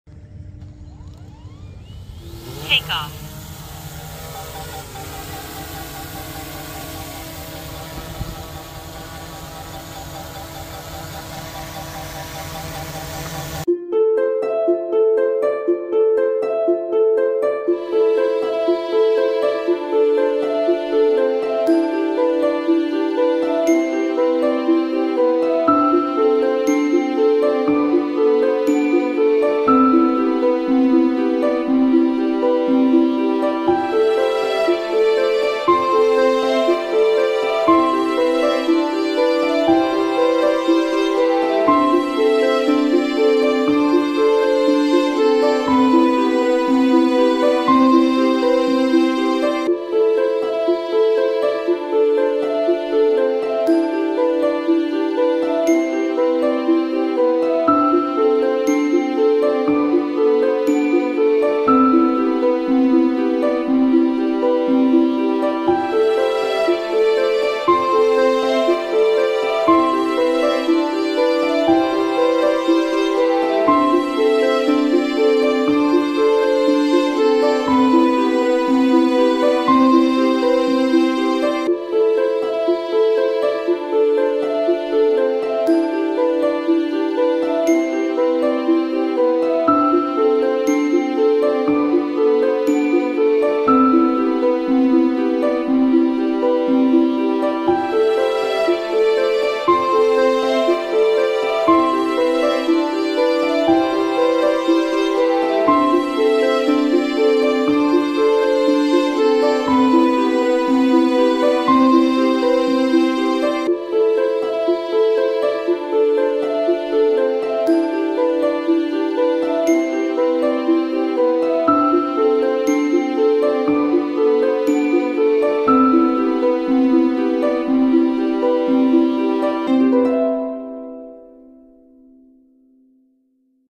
Flew Air 3 over to the SS Selma
My first flight over to the SS Selma with the Air3 $DjiDrone